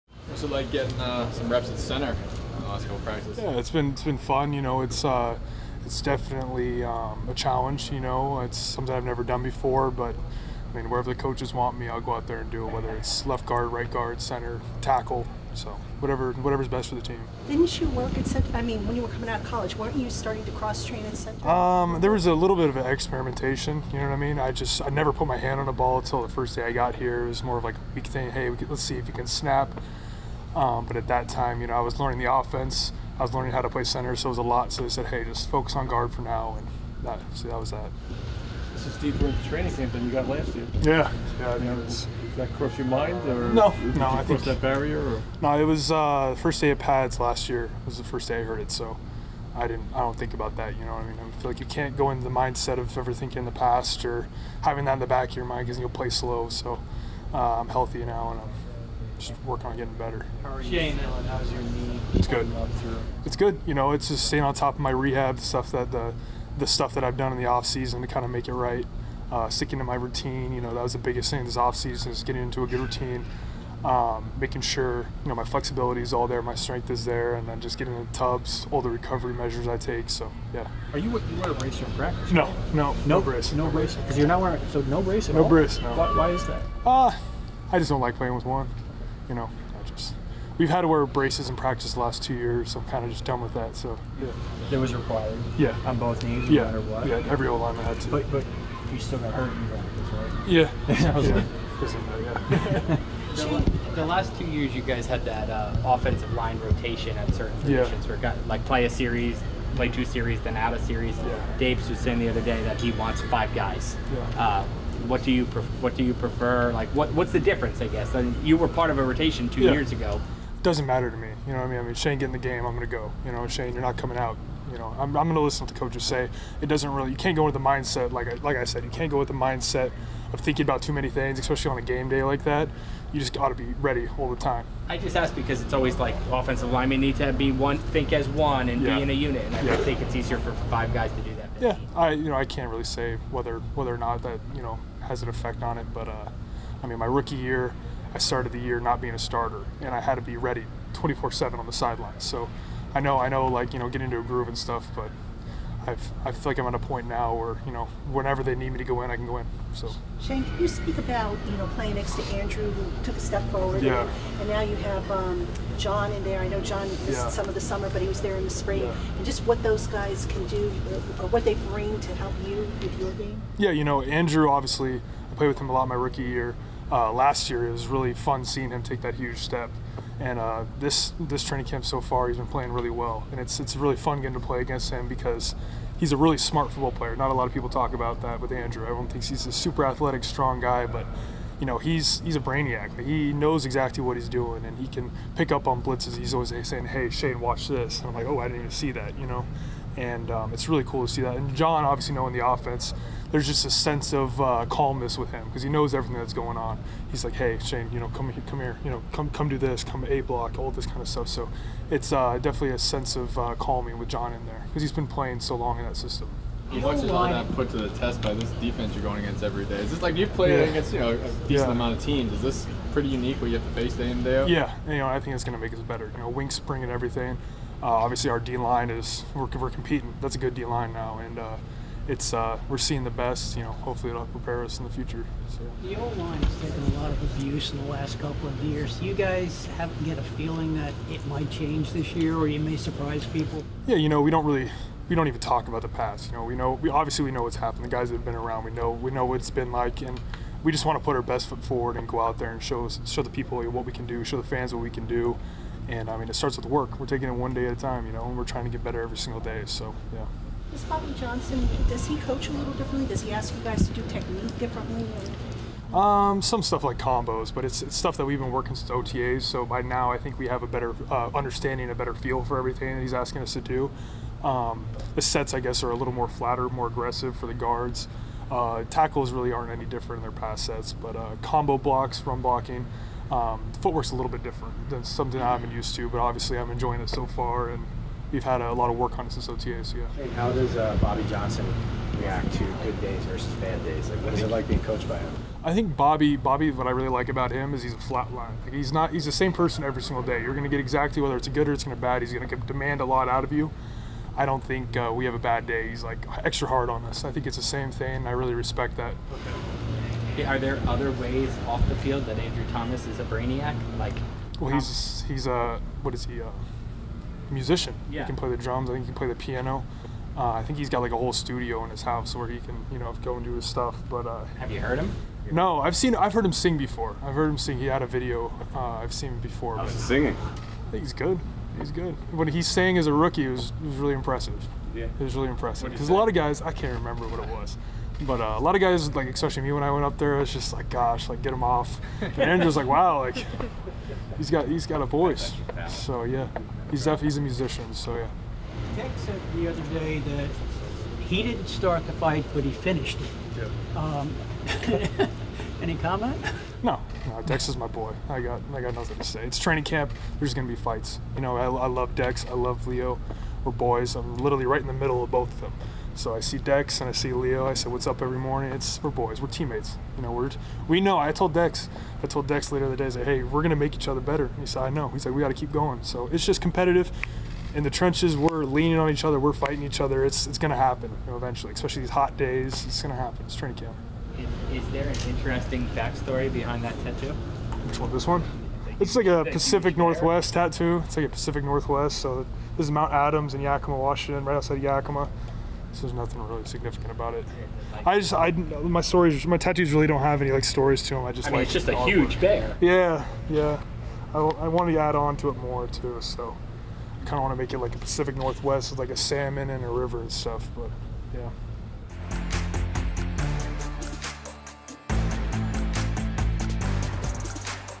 Shane Lemieux on Andrew Thomas: 'He's a brainiac' | New York Giants
Offensive lineman Shane Lemieux speaks to the media after Wednesday's training camp practice, presented by Ford.